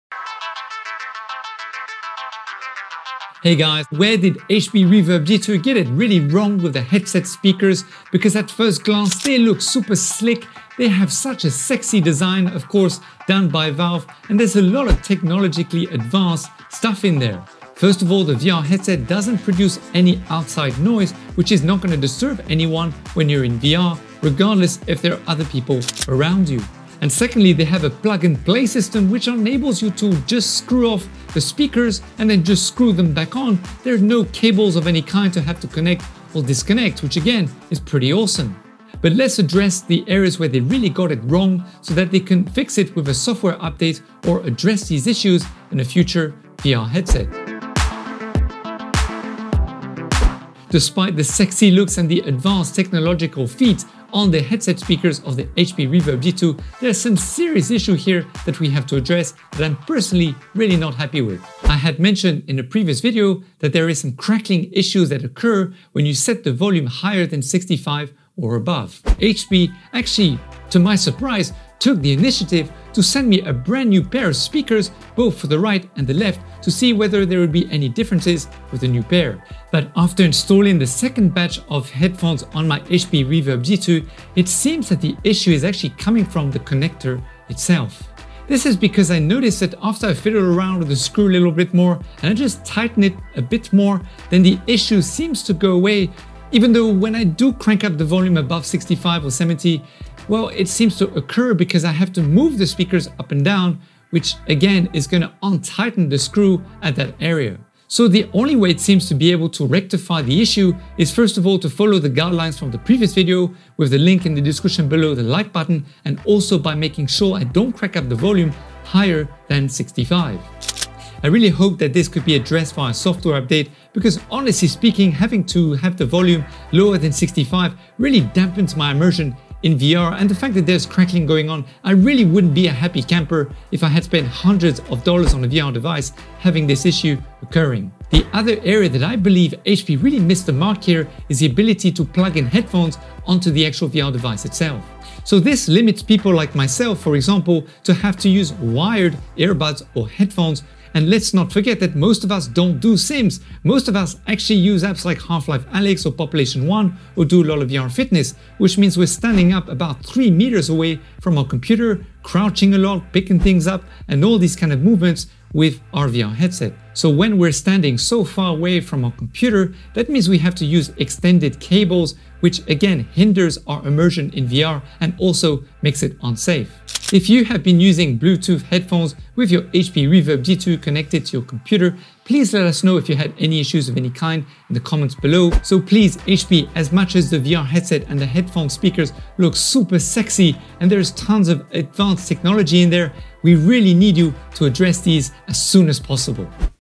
HP Reverb G2 FAULTY VALVE HEADSET SPEAKERS? Where did they go wrong?
In today's video, we look at the complaints that have been made recently about people's hp reverb g2 valve headset speakers if they are indeed faulty as we have also received 2 pairs, and both had issues. We believe the issue is at the connector issue. We also explore if it would be better to add a headphone jack in the next VR headset also.

HEADPHONES & CONTROLLER
Audio Technica noise-cancelling headphones

BATTERIES
1.5V rechargeable AA batteries

4K HDMI cable